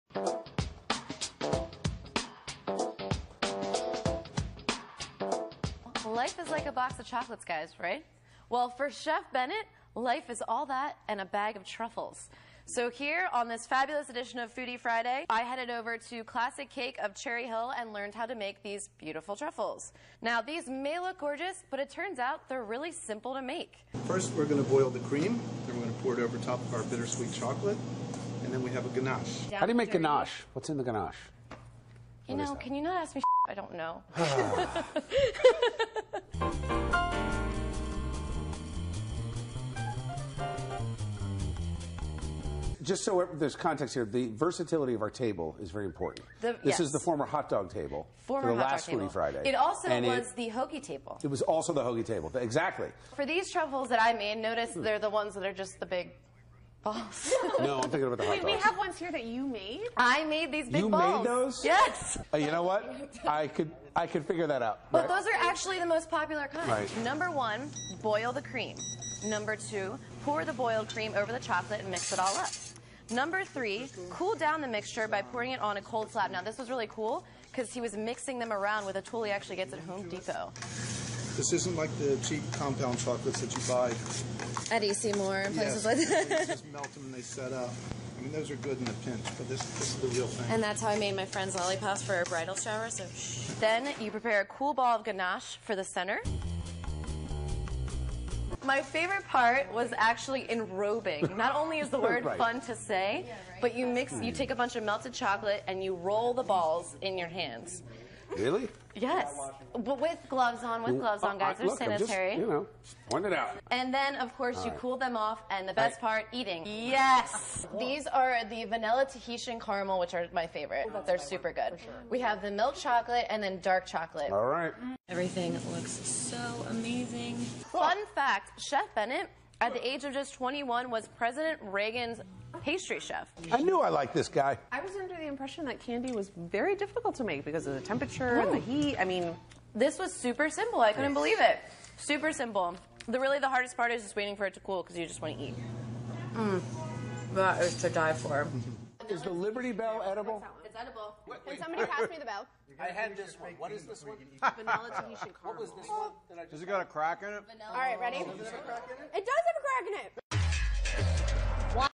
All that and a box of truffles
Life is like a box of chocolates, right?  Well for Chef Robert Bennett, it's all that and a bag of truffles.